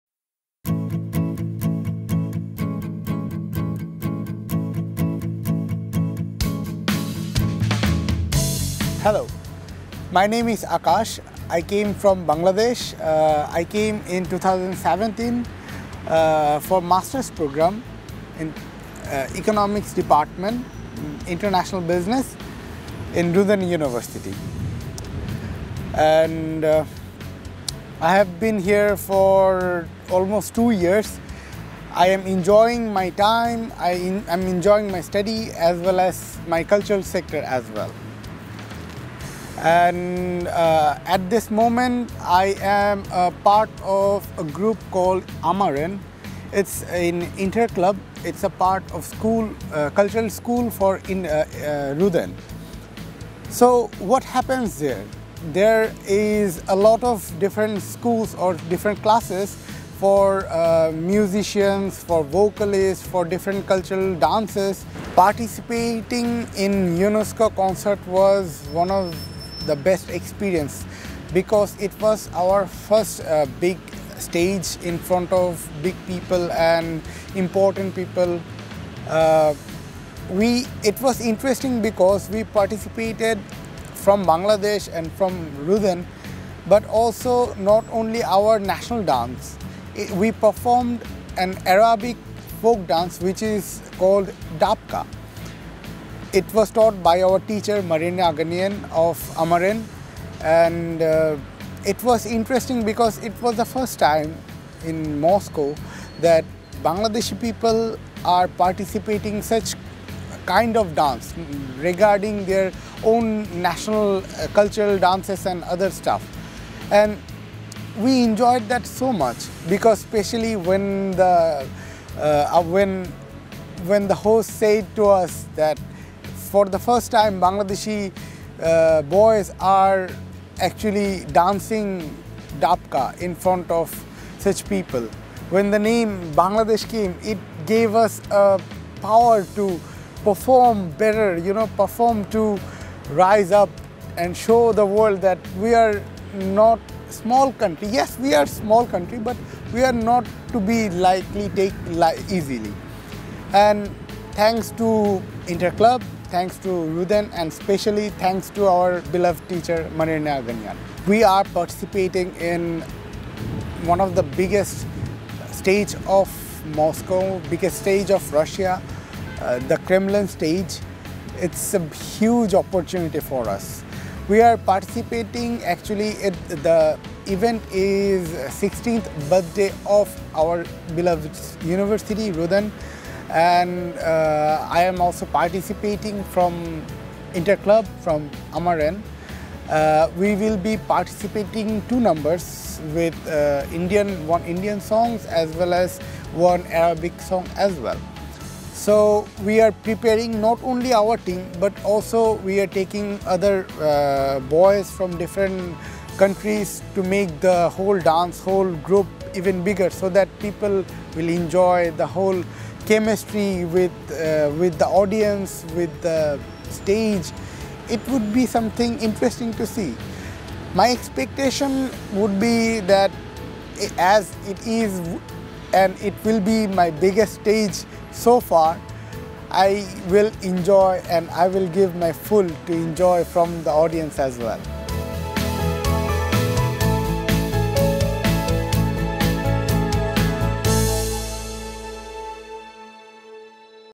Talented RUDN: Mohammad Maksudur Rahman
Talented RUDN: Mohammad Maksudur Rahman, master student of economics department in international Business of  RUDN University, Bangladesh.